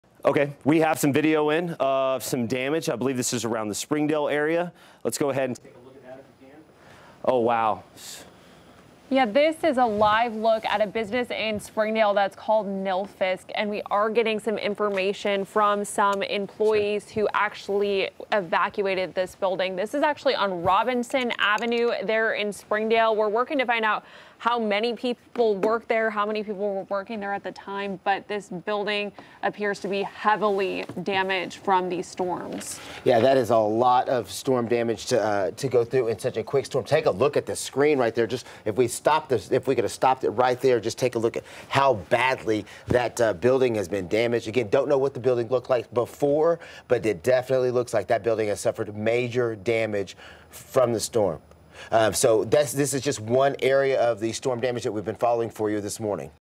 Nilfisk-Advance on Robinson Ave. in Springdale heavily damaged by storm
A business on Robinson Ave. in Springdale suffered heavy storm damage March 30